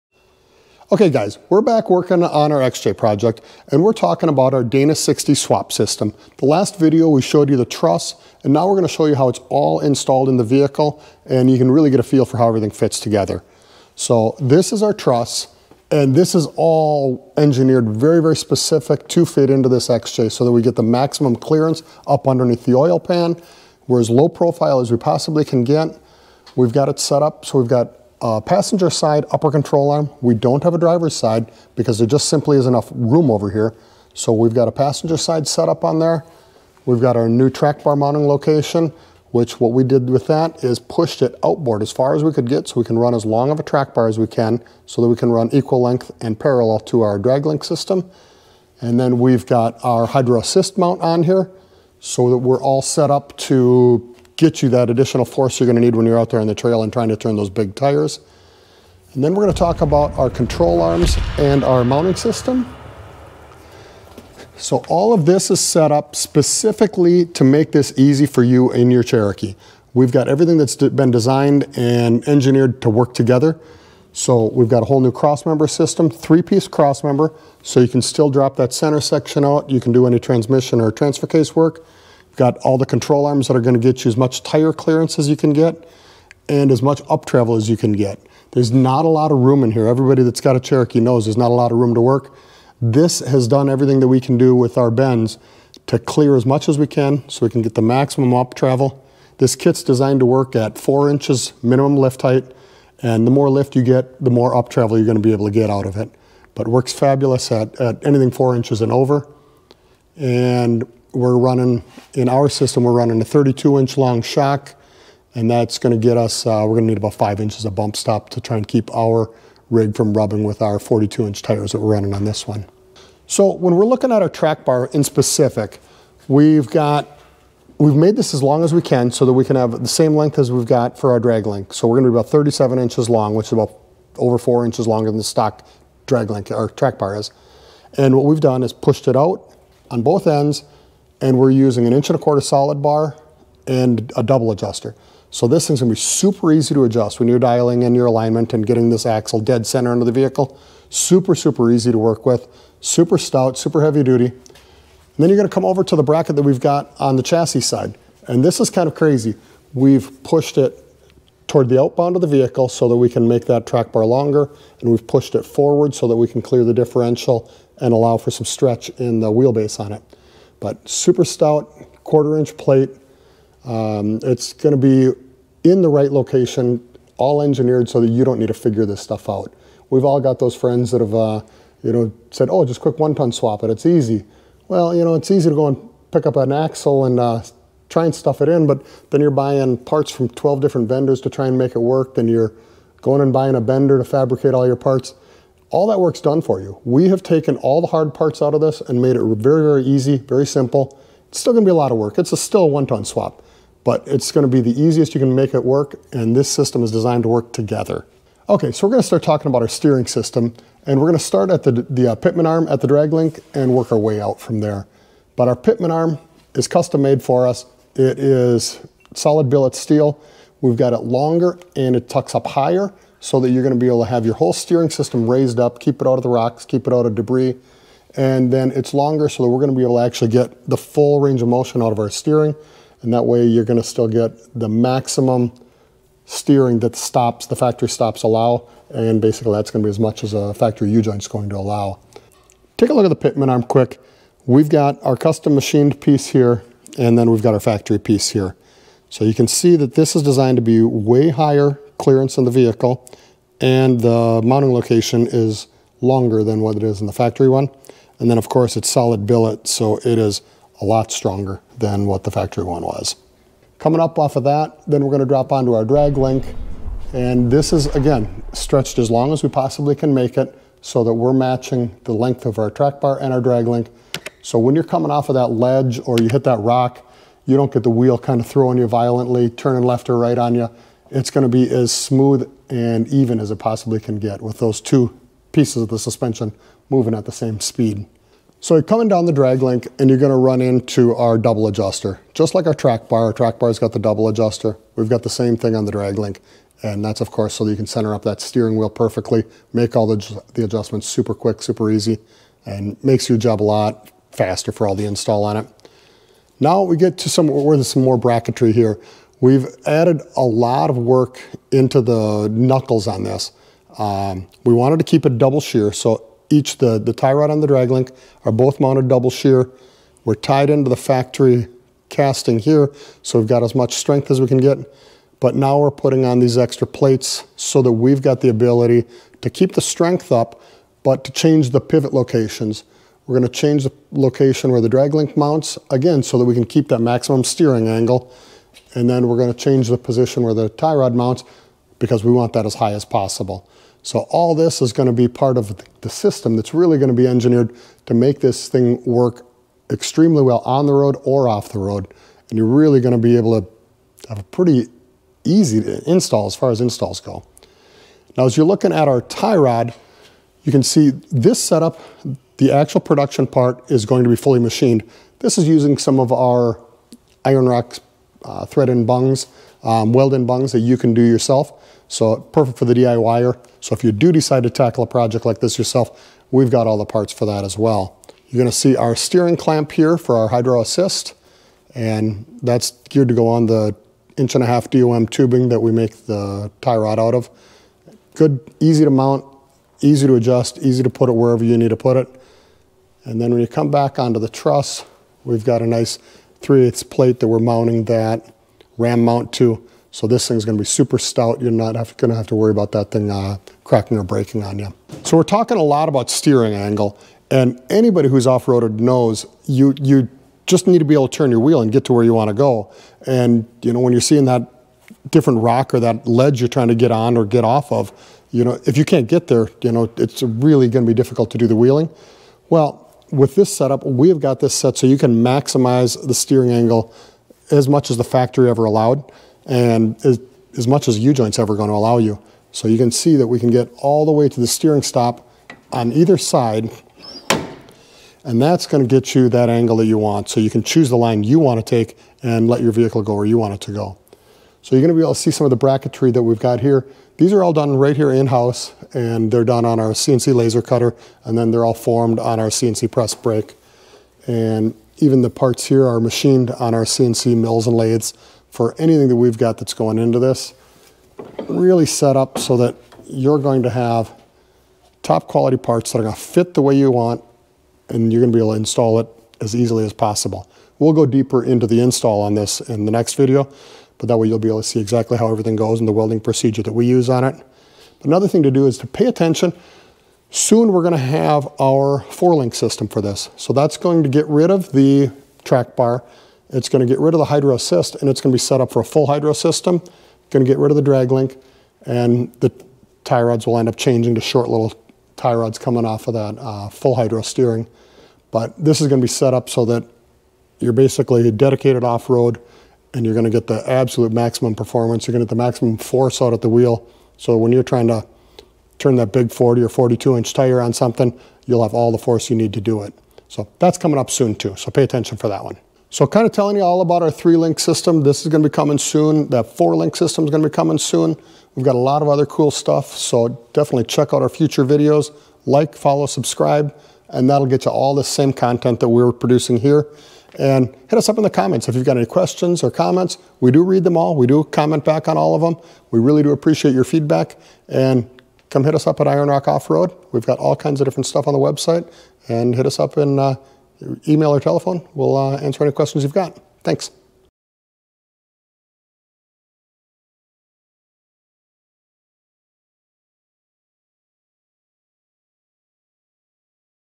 COMPLETE System - XJ 1-Ton Steering & Suspension - It all works together!
Are you looking for a COMPLETE solution to put a Super Duty Dana 60 front axle under your XJ?  Iron Rock Off Road has engineered a swap kit that makes it EASY! We covered everything: bump stops, brake hoses, steering, it's all made simple with this kit!

This is our 3-link version of our Dana 60 front axle swap kit. It includes an optimized track bar mount, shock mounts, and a ram assist mount incorporated into the truss design.  We have designed a high clearance 3-link suspension system and a full steering system that is all engineered to work together as a complete unit. We worked hard to get the maximum uptravel, flex, and steering angle out of this 1-ton axle swap, so you won't be held back on the toughest trails!

We will also have a triangulated 4-link truss with a full hydro steering setup coming soon!